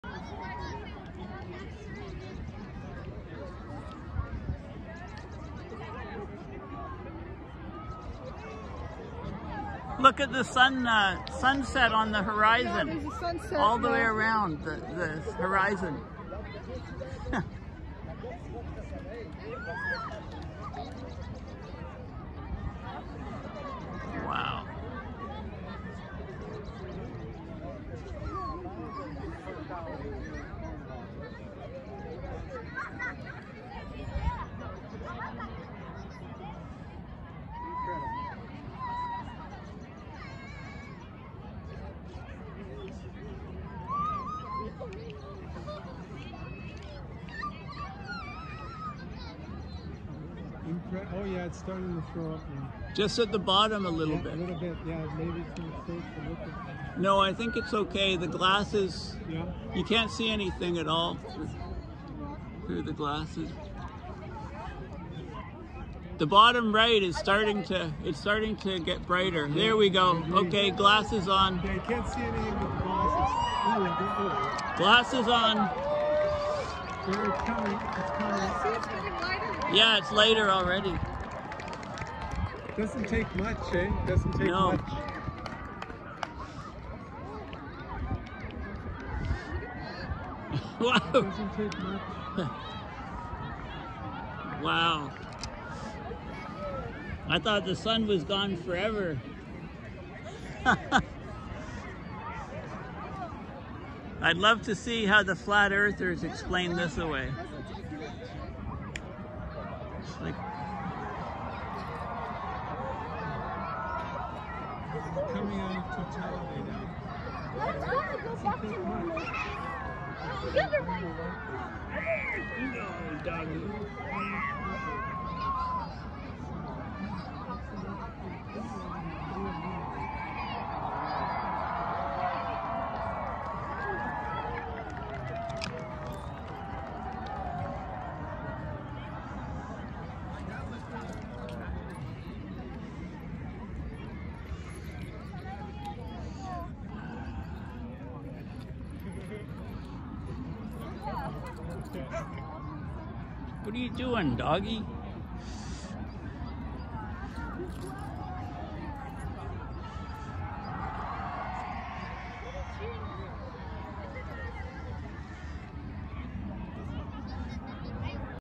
Solar Eclipse Totality in Cornwall, Ontario April 8, 2024
I drove about an hour south from Ottawa today to Cornwall Ontario to see totality of the lunar eclipse of the Sun. Totality lasted 2 minutes in my location, and the whole thing lasted 2 hours and 24 minutes.

It was awe inspiring, life-changing, and very emotional and profound to experience it. Totally different from even a 99% partial eclipse. I may have to add eclipse chasing to my hurricane and tornado storm chasing!!

It was really freaky. The birds all went quiet, it got much darker than I expected, and the temperature dropped 2 to 3 C in minutes, taking almost an hour to recover. During the darkness, scanning 360 degrees it looked like sunset on every horizon.

I took some Cineplex 3D plastic glasses, removed the polarizer lenses, and used electrical tape to attach my paper solar eclipse glasses to the durable frames. Worked a charm.

Newton was freaked out by my glasses and the eclipse, but loved chasing the stick I threw far out into the St. Lawrence River. He almost pulled me into the river once, when he spotted some Canada Geese swimming. It was totally unexpected, since I had the eclipse glasses on and was captivated by the celestial spectacle.

I was relieved when the Sun came back, since I had envisioned global chaos within hours, agriculture failure within days, and oceans freezing over within weeks to months, not to mention a possible Zombie Apoc-Elipse, if the Sun had not reappeared.

Sincerely,
Paul